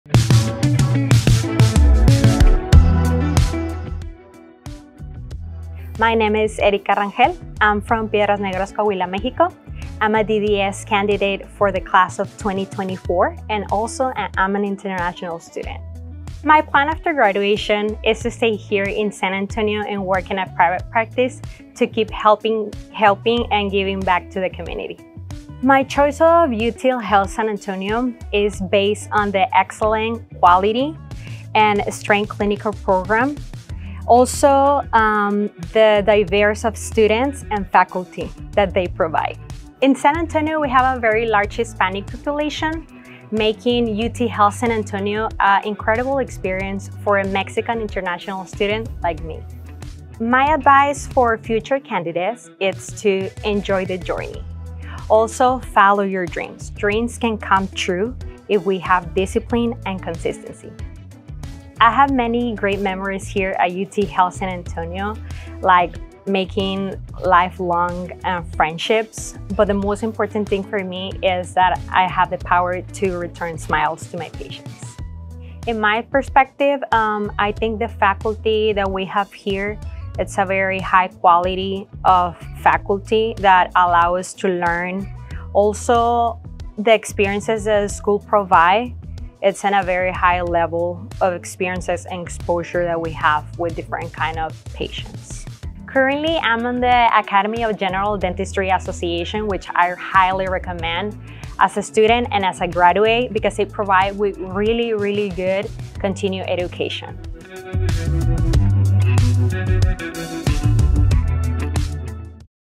School of Dentistry at UT Health San Antonio 2024 Graduation Spotlight: Erika Selina Rangel Juarez
Countdown to graduation spotlight: Erika Selina Rangel Juarez is graduating with a Doctor of Dental Surgery (DDS) degree from the  School of Dentistry's International Dentist Education Program (IDEP) at UT Health San Antonio!

Congratulations to Erika and all our School of Dentistry’s DDS Class of 2024! 👏 🙌 🎉